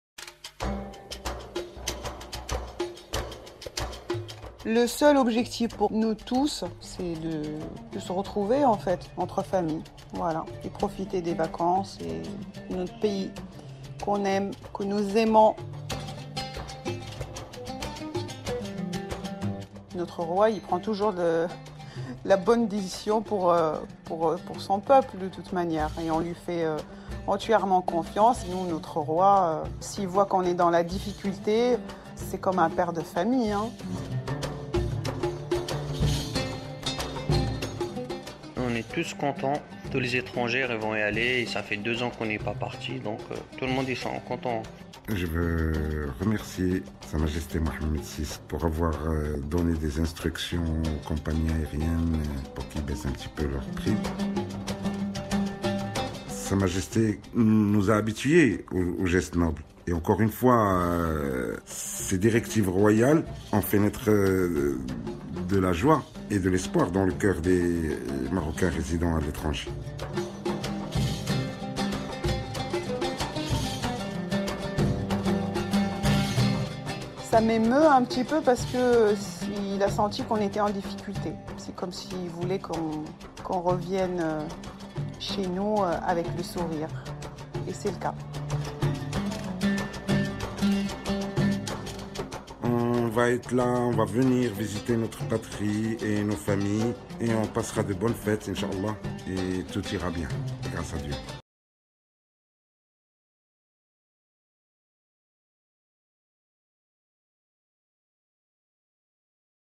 La parole aux Marocains du Monde qui s'apprêtent à rentrer dans le Royaume
A l'occasion du coup d'envoi de l'opération Marhaba 2021 et de l'initiative du Roi Mohammed VI pour faciliter le retour au pays des marocains établis à l'étranger, Medi1 vous propose cette semaine un nouveau rendez-vous. "Bienvenue chez vous" : la parole aux Marocains du Monde qui s'apprêtent à rentrer dans le Royaume.